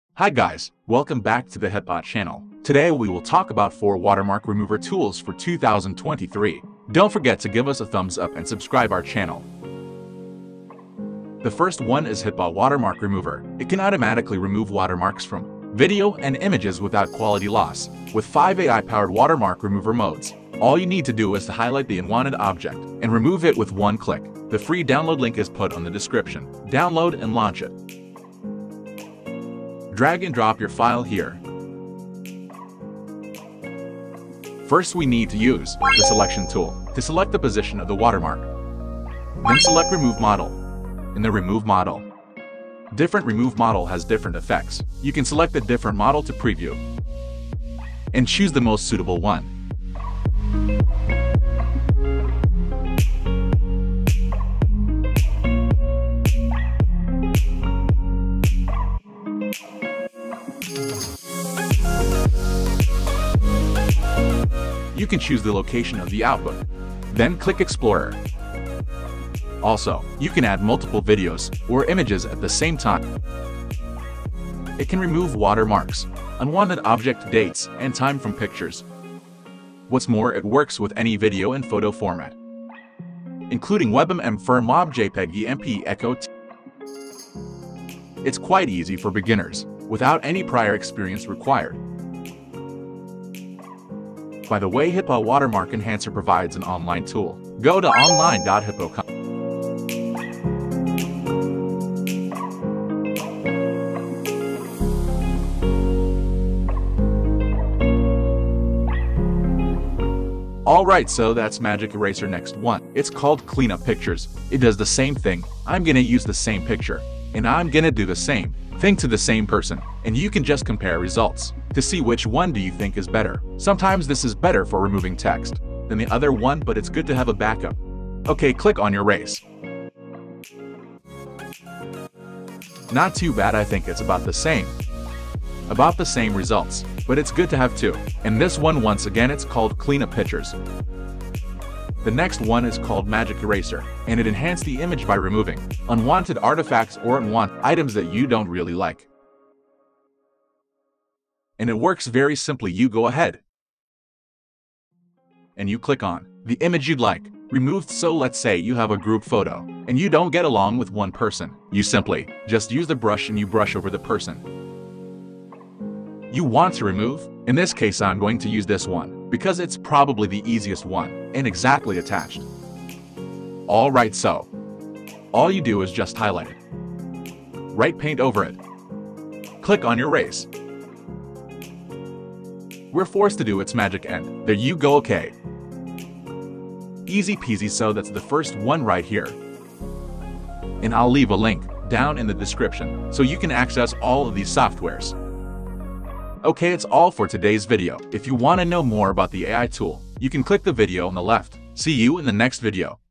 Top 3 Ai Watermark Remover Online & Offline [2023 Tutorial]
Chapter
00:00 Intro & Preview
00:13 HitPaw Watermark Remover
01:50 Cleanup Picture
02:21 Magic Eraser